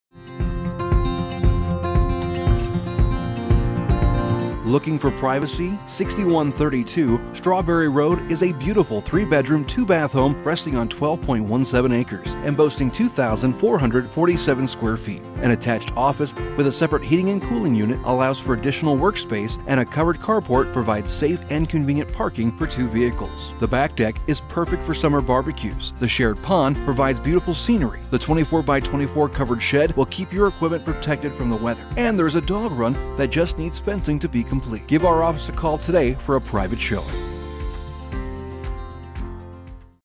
6132 Strawberry Road - Quiet Country Living!
Looking for privacy? 6132 Strawberry Road is a beautiful 3 bedroom, 2 bath home resting on 12.17 acres and boasting 2,447 square feet. An attached office with a separate heating and cooling unit allows for additional work space, and a covered car port provides safe and convenient parking for two vehicles. The back deck is perfect for summer barbecues, the shared pond provides beautiful scenery, the 24 by 24 covered shed will keep your equipment protected from the weather, and there is a dog run that just needs fencing to be complete! Give our office a call today for a private showing!